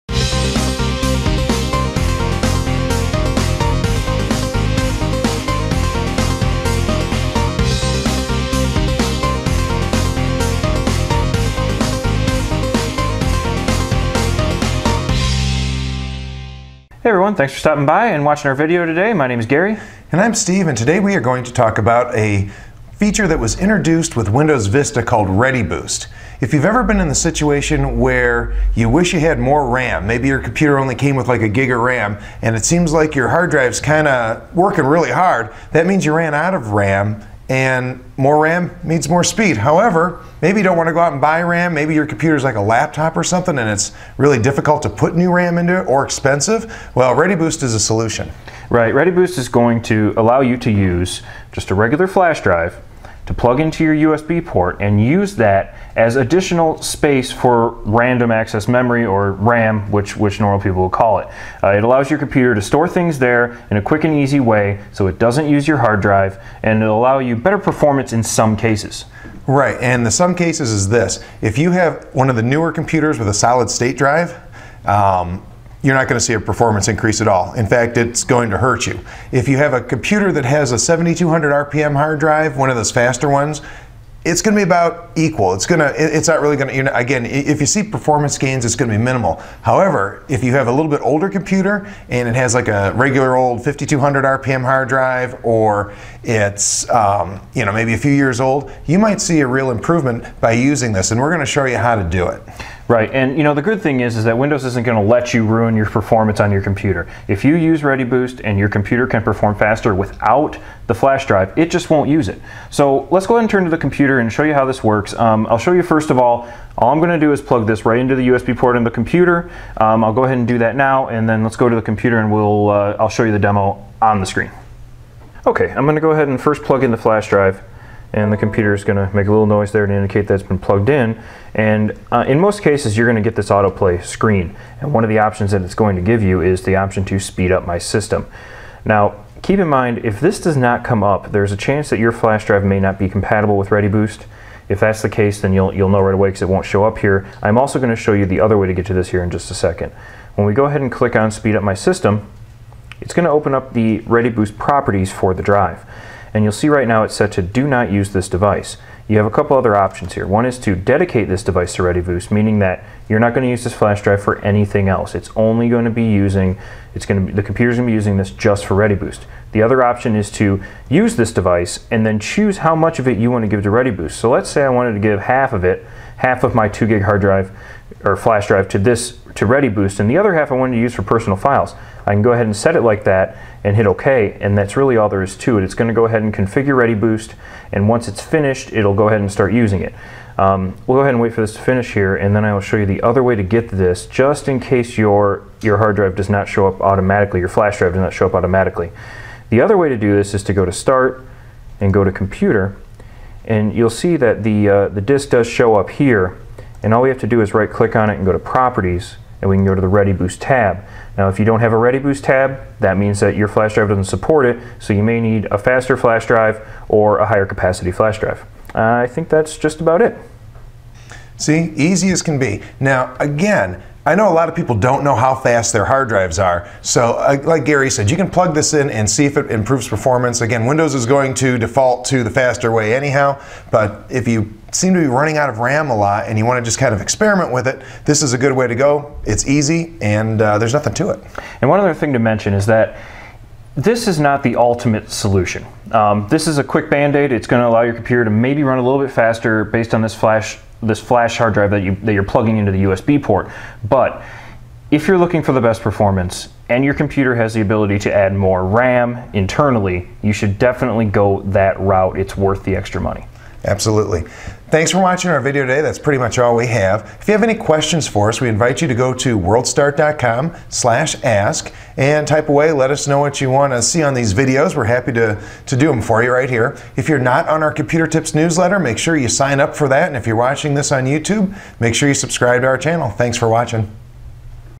How To Use Windows ReadyBoost
Steve and Gary from Worldstart.com explain the pros and cons of Windows ReadyBoost